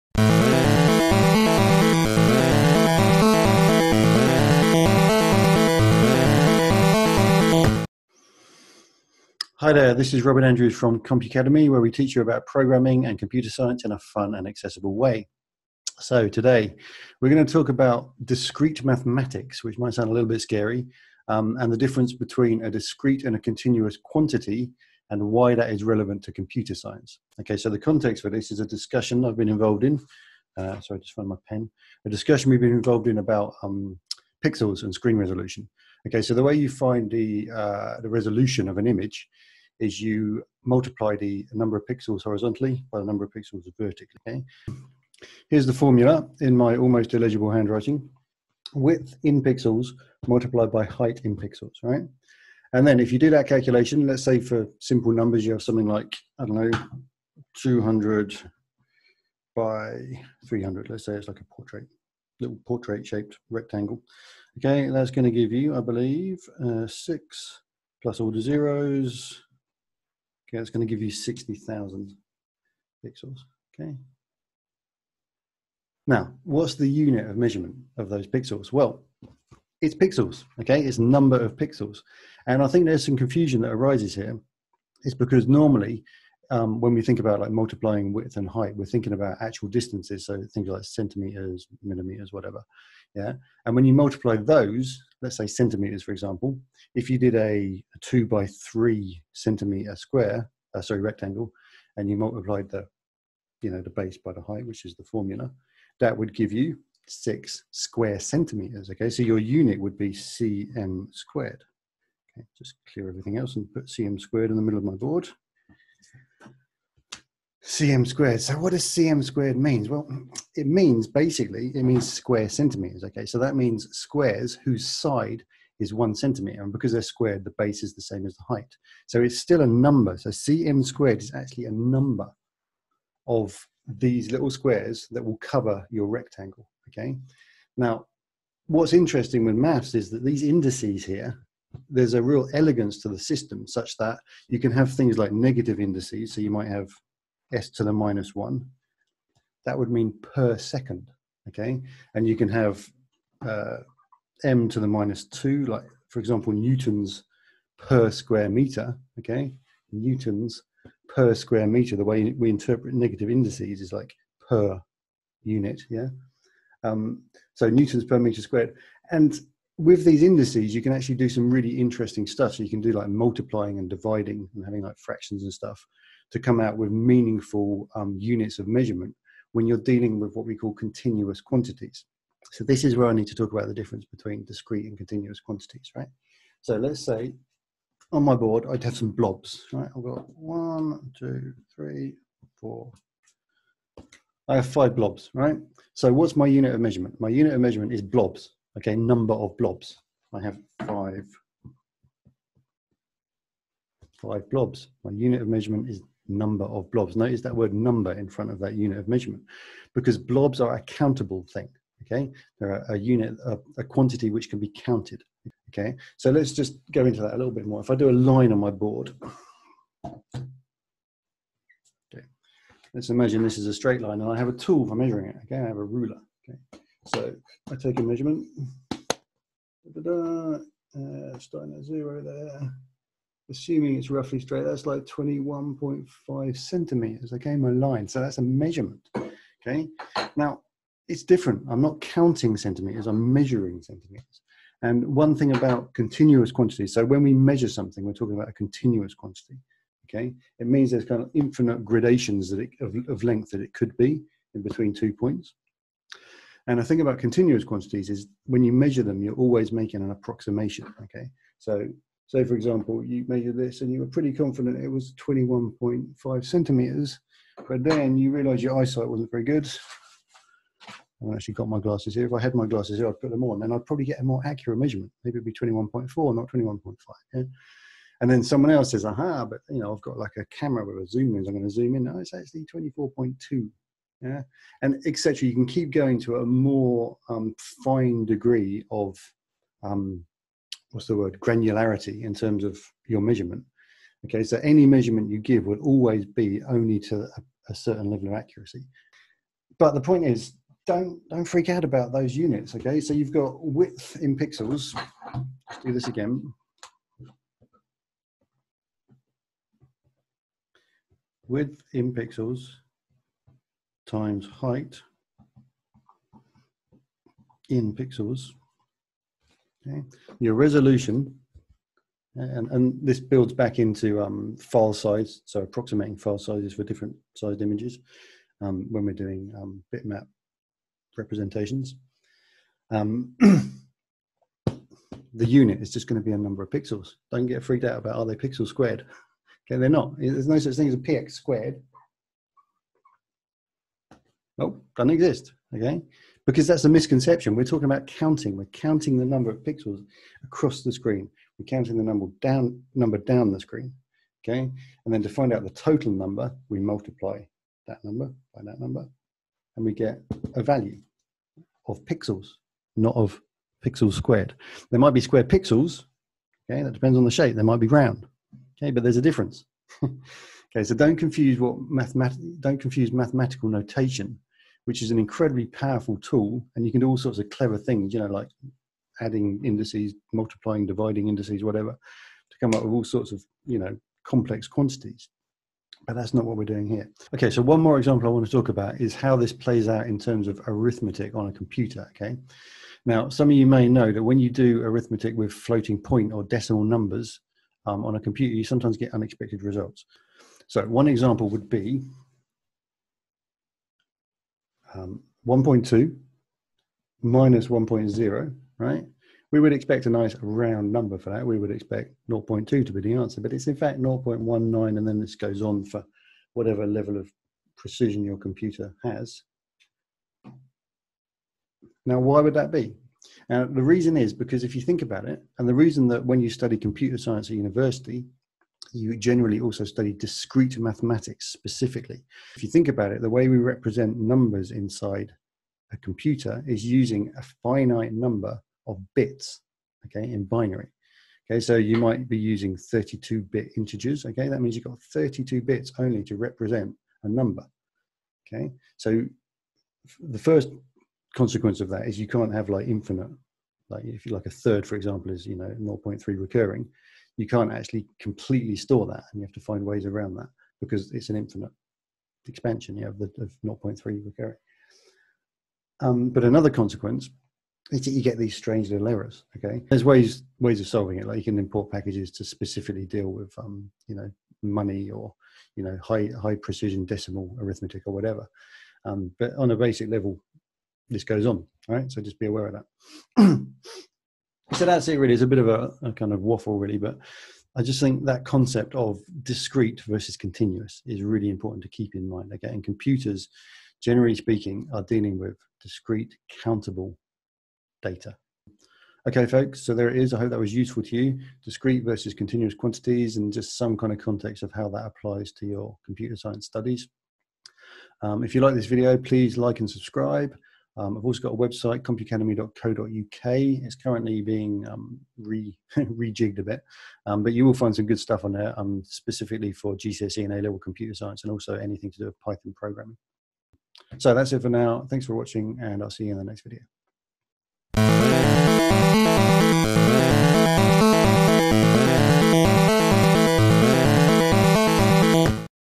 Discrete Quantities vs Continuous Quantities in Computing
This is a video about the difference between discrete and continuous quantities and why that's important in computing.

"Pixel resolution: The term resolution is often considered equivalent to pixel count in digital imaging, though international standards in the digital camera field specify it should instead be called "Number of Total Pixels" in relation to image sensors, and as "Number of Recorded Pixels" for what is fully captured" - (Wikipedia)

Please note in this video I refer to the resolution of an image using the "pixel resolution" definition given above. some confusion can arise when people mix this up with "spatial resolution", for example "pixels per inch" or "dots per inch".

Topics covered:

- Calculating pixel resolution of images
- What is a discrete quantity?
- What is a continuous quantity?
- How do discrete quantities relate to computing?
- Why computers are bad at decimals

The Compucademy website has many articles and resources for learning Python programming and Computer Science in a fun and accessible way